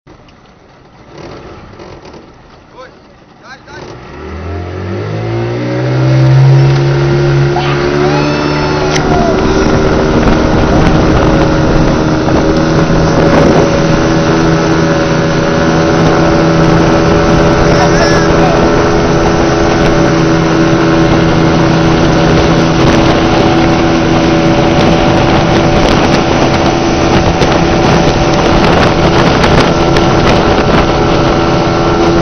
Surf board na Ilha do Cardoso
Primeira vez que fiz surf board sem ser no mar. É outra história esquiar num rio liso... mas liso messssmo!
Detalhe para a saída, correndo de cima do trapiche e pulando direto na prancha!
Ah, e na próxima, com alça na prancha dá até pra voltar na última manobra ali hehehe!